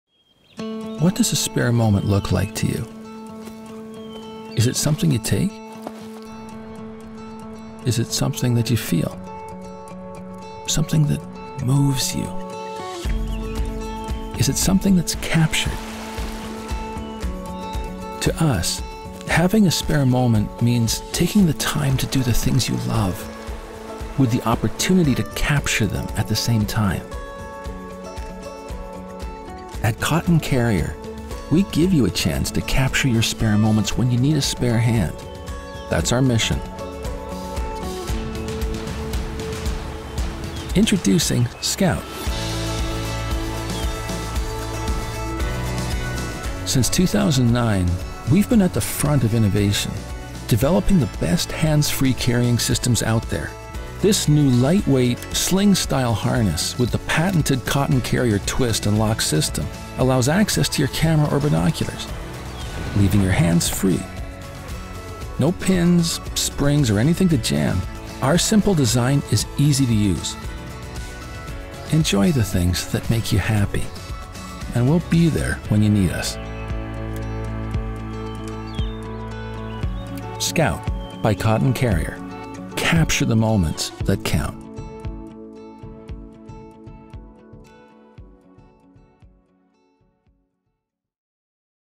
Cotton Carrier Presents: The Skout - Camera and Binocular Harness
After 9 years in business we launched our latest in camera and binocular carrying innovations. The NEW sling style harness from Cotton Carrier, The Skout.